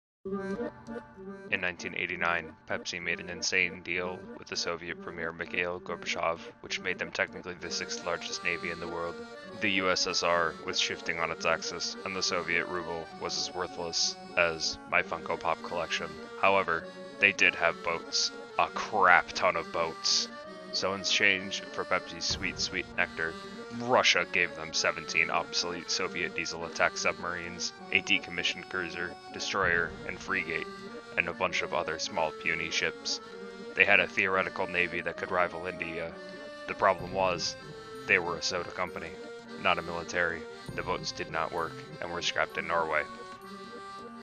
How Pepsi Acquired a Navy
A quick recap on how Pepsi made a deal with the Soviet Union and acquired the 6th largest navy in the world.
Special thanks to @caldenza for doing the voice work
Edited and written by yours truly.